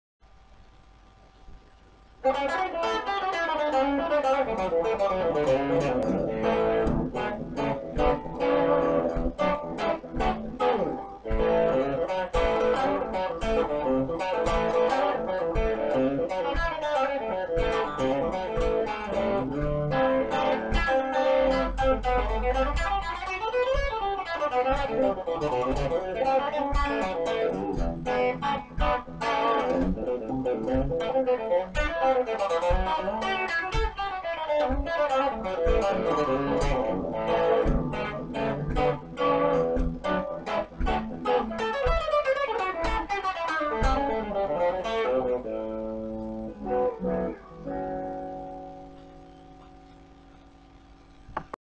Country blues study.
Here's a little study I wrote that contains a number of interesting licks. I only play thing once round here but of course you can repeat it as many times as you like and improvise variations if you feel so inclined.